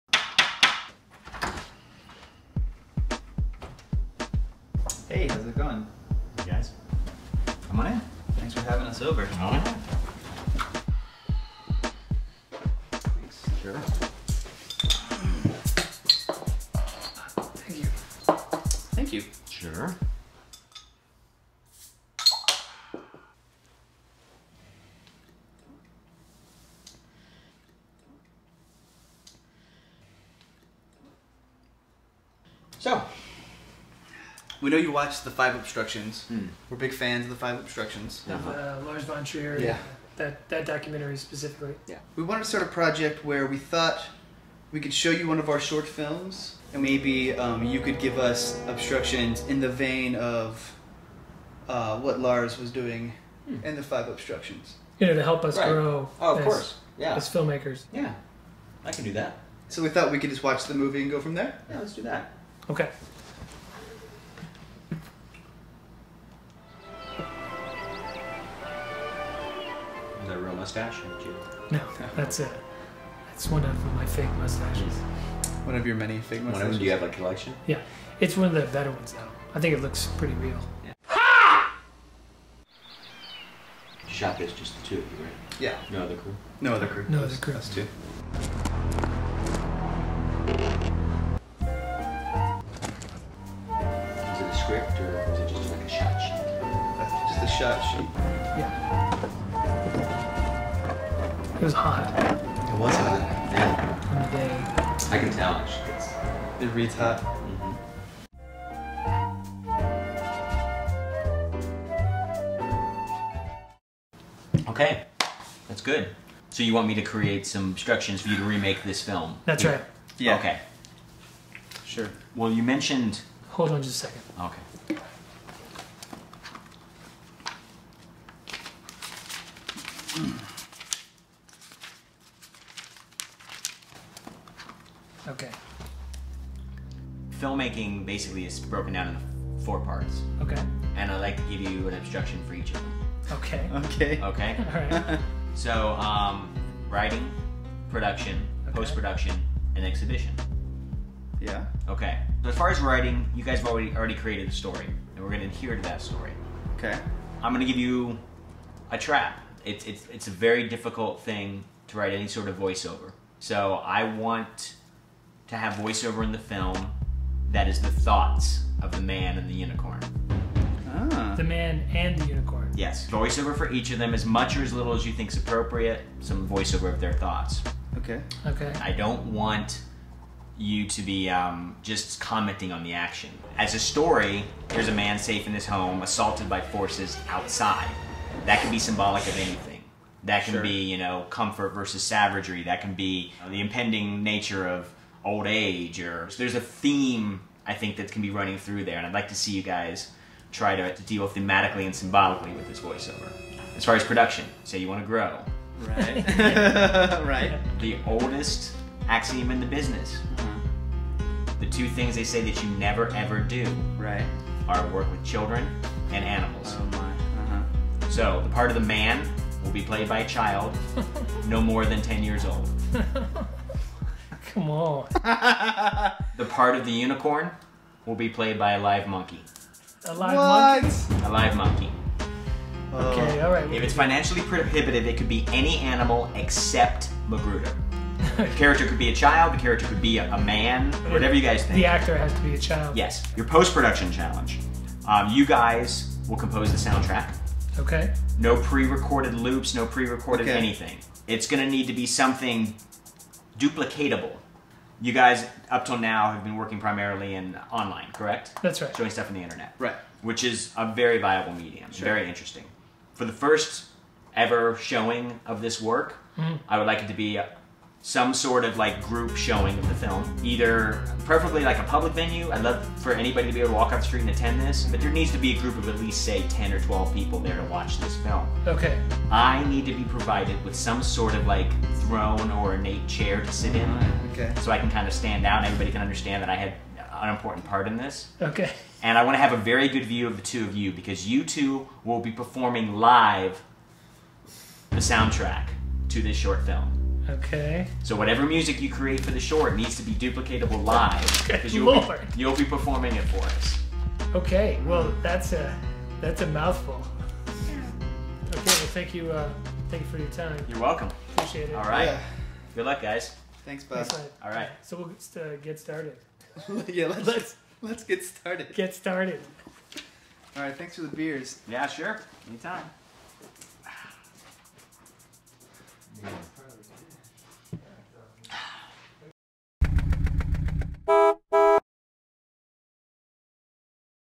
Unicorn Obstructions: Bob DeRosa - CarBoy Films
Screenwriter Bob DeRosa give us obstructions to remaking our short film "Unicorn".